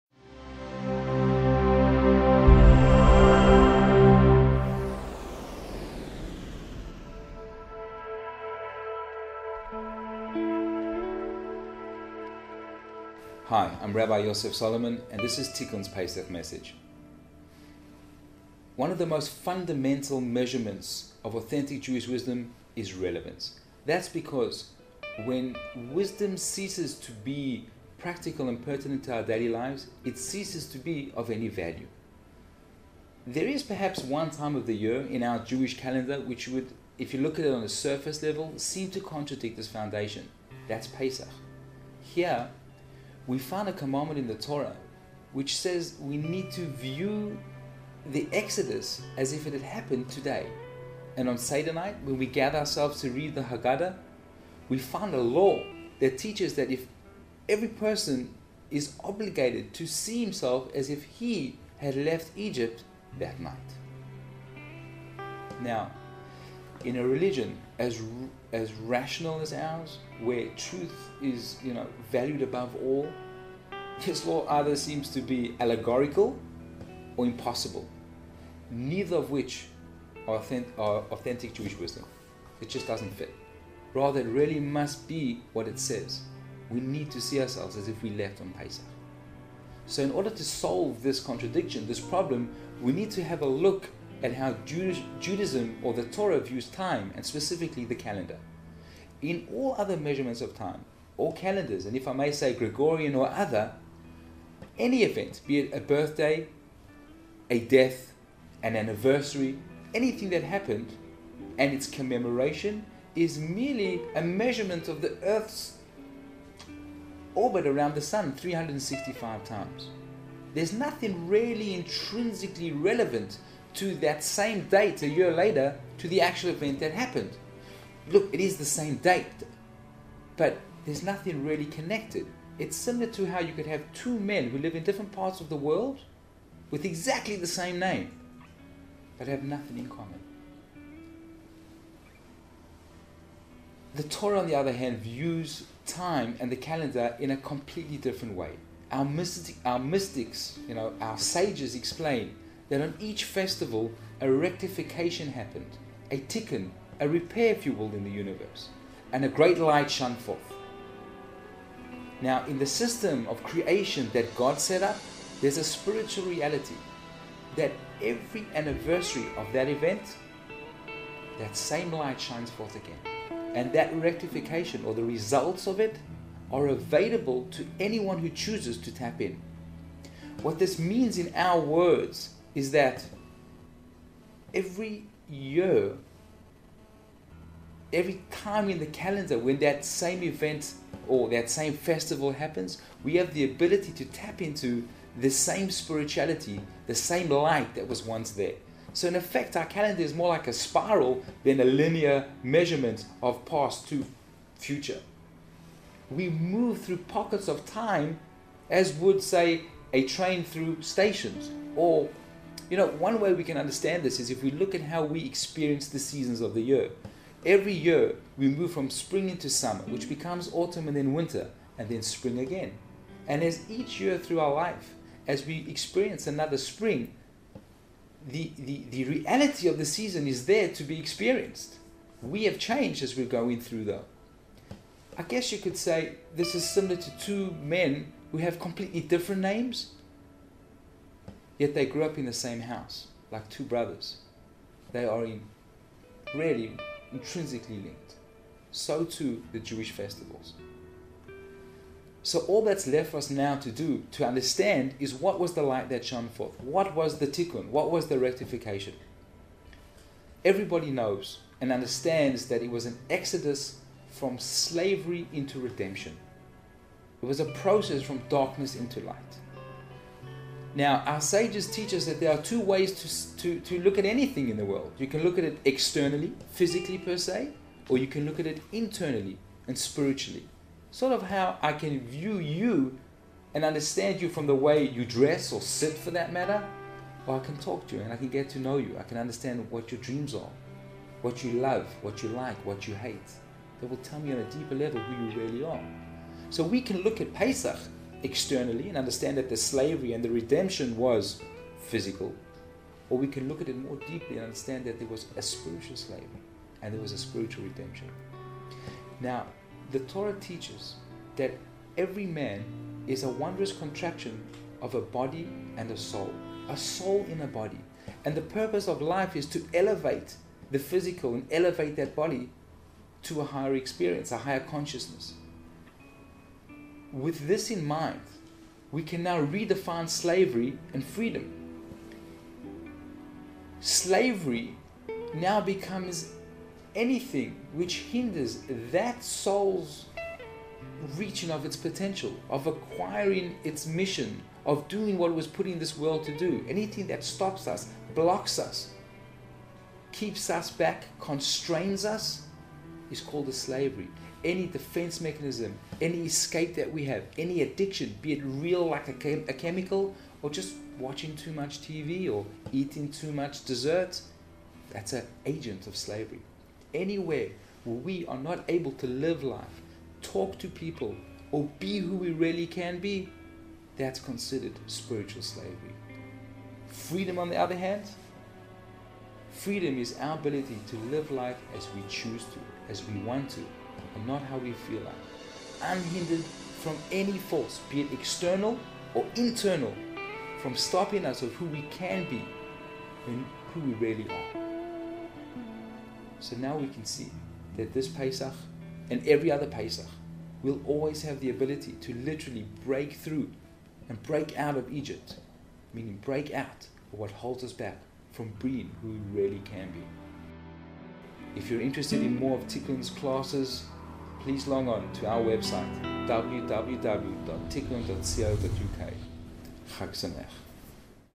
Pesach Message
Break free this Pesach - Rabbi Yosef Solomon

Pesach is about freedom. No human being is completely free from their personal desires, from their ego or passions. Each and every one of us has our own barriers that hold us back from being who we want to be. Pesach is a time to break free from these shackles. It's a spiritual time when these bonds are easier to break. Take a few moments to watch our Pesach video and utilise the meaningful change that Passover offers. We hope to see you at one of our programmes soon.

Tikun wishes you a meaningful and uplifting Passover. Chag Sameach